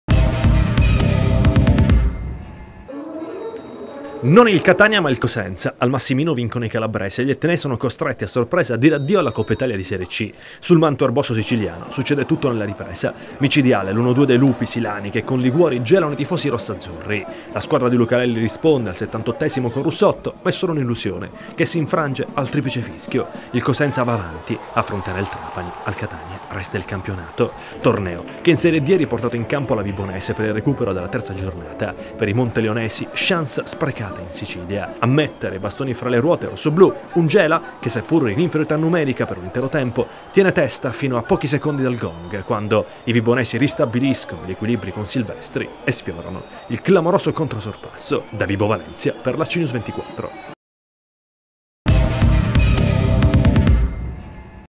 Cosenza avanti in coppa. Vibonese stop a Gela
E intanto ieri sera è sceso in campo il Cosenza che batte il Catania e prosegue la sua avventura in coppa Italia. In campo anche la vibonese in Serie D per il recupero della terza giornata di campionato. A Gela finisce 1-1.